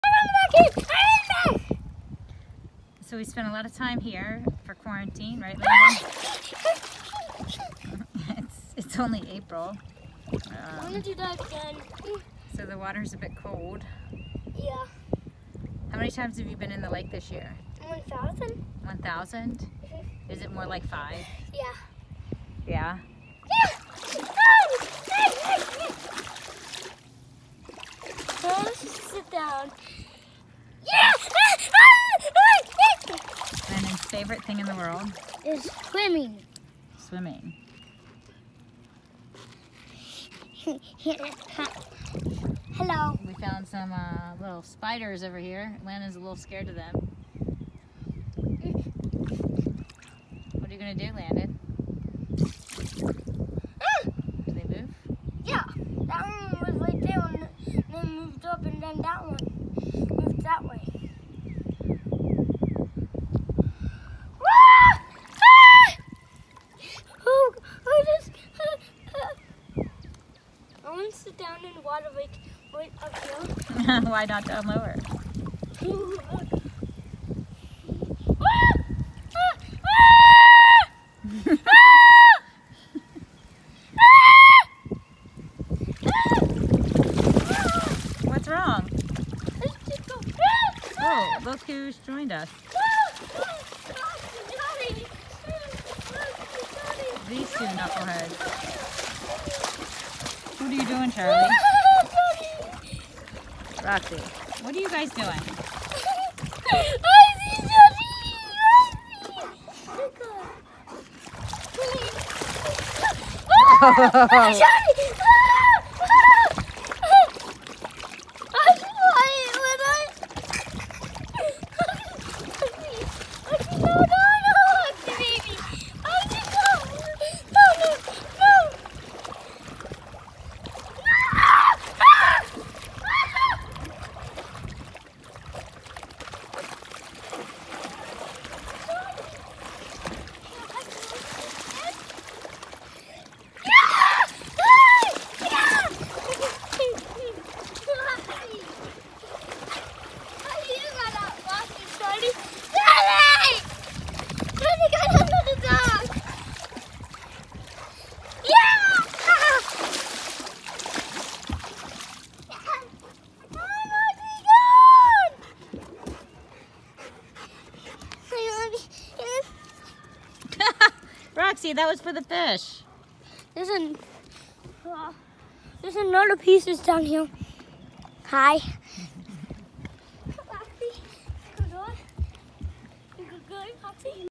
Landon taking a Dip in the Lake
Wednesday of week 4 of quarantine. Probably Landon’s 4th time in the lake this year... on purpose!